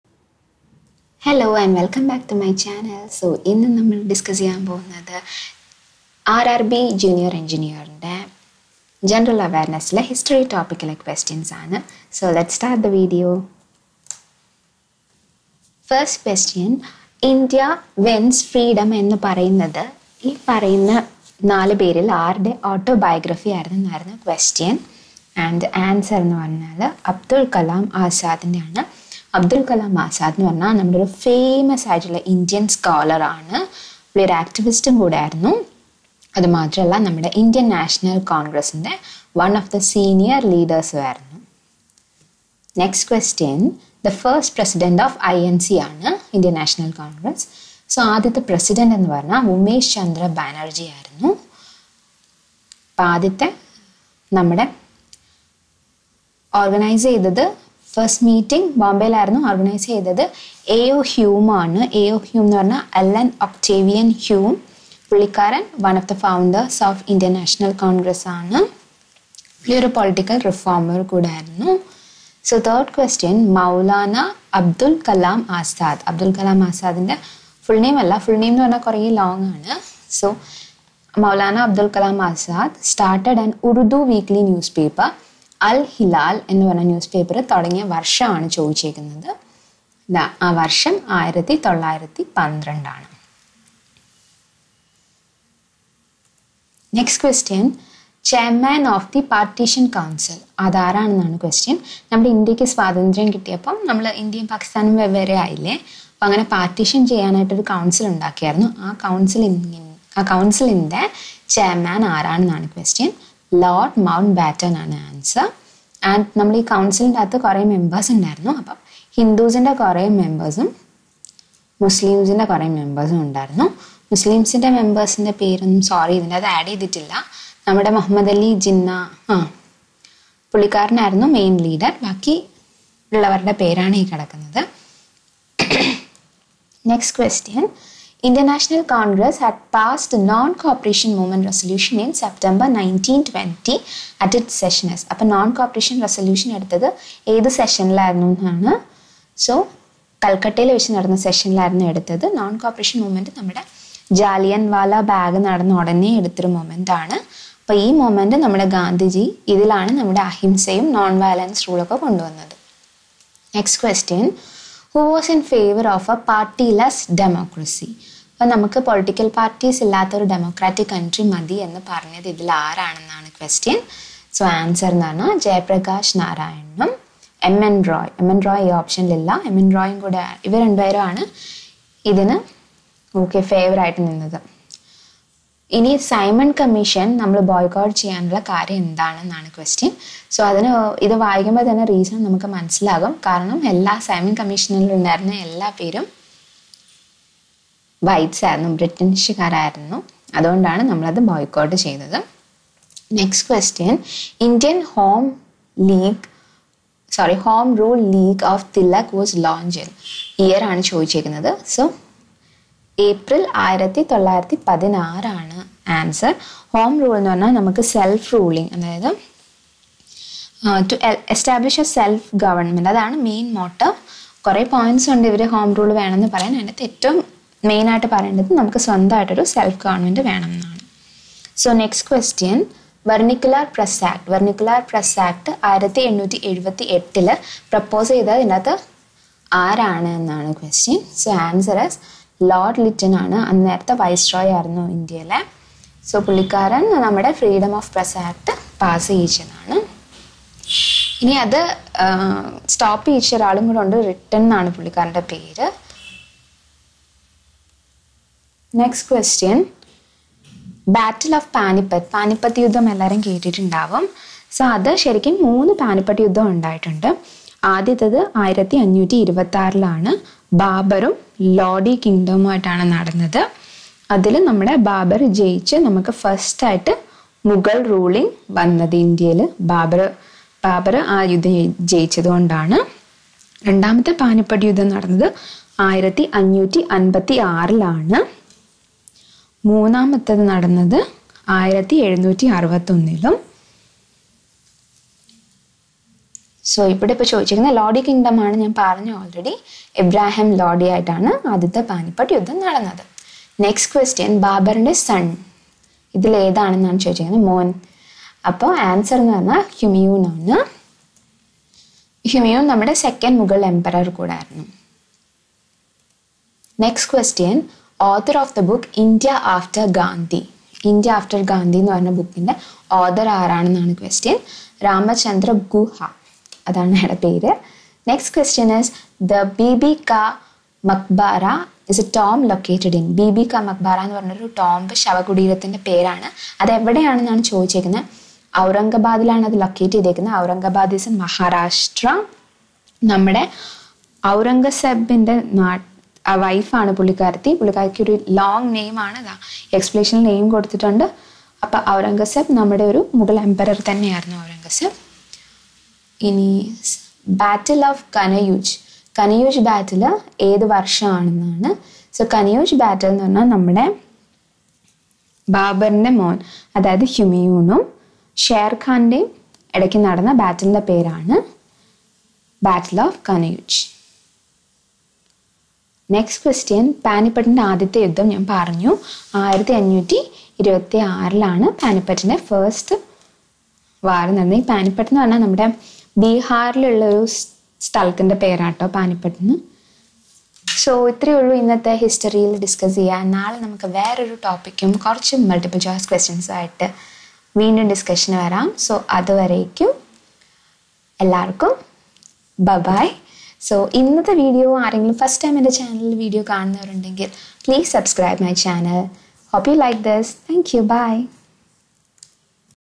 RRB JE 2019 DAY 2||GENERAL AWARENESS|| Indian History||Malayalam
The video covers the important topics for RRB JE 2019 along with MCQ questions on INDIAN